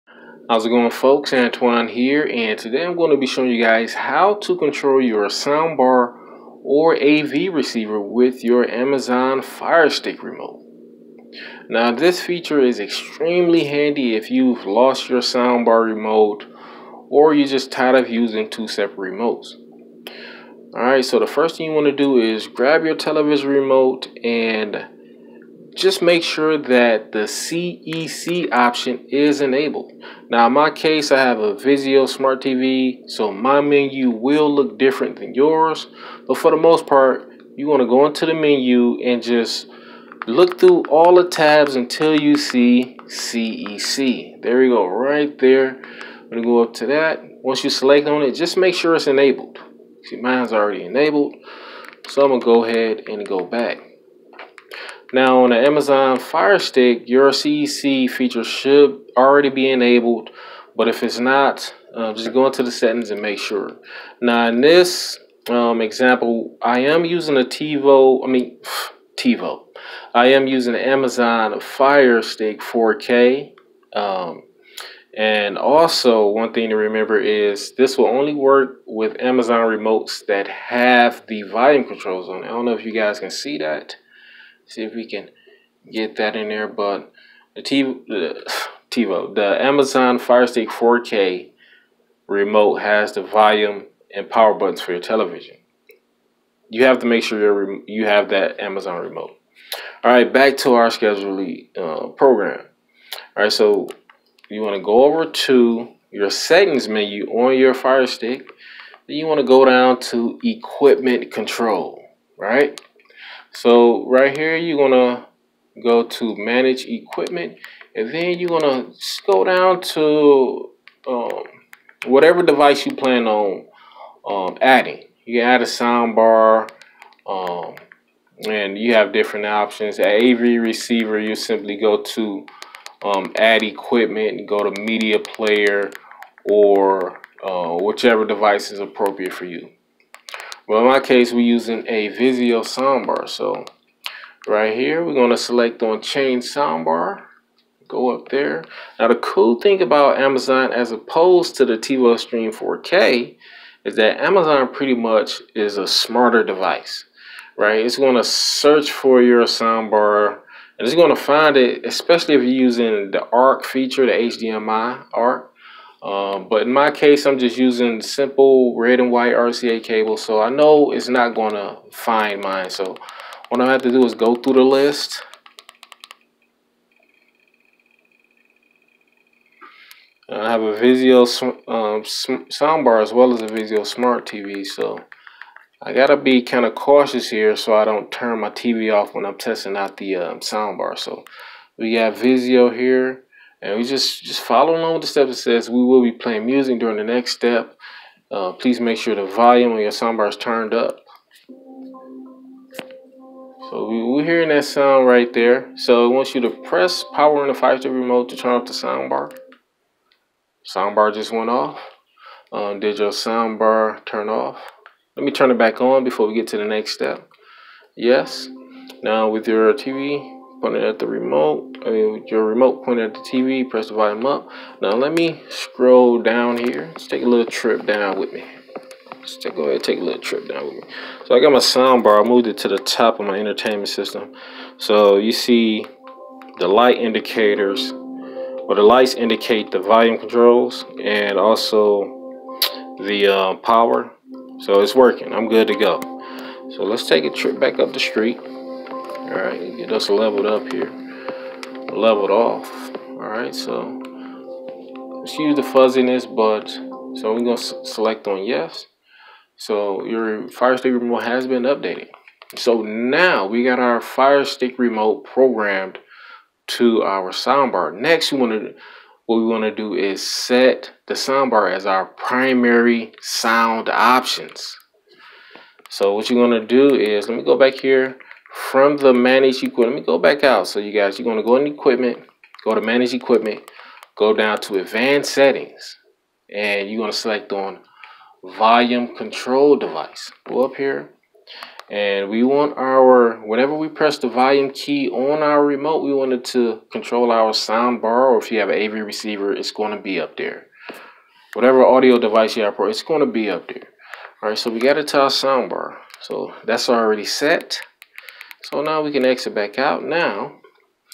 How to Control your Soundbar with a Fire Stick Remote
Learn how to setup your Amazon Fire Stick remote to control your soundbar or A/V Receiver. This helps get rid of extra remote controls and is also useful if you don't have your soundbar remote.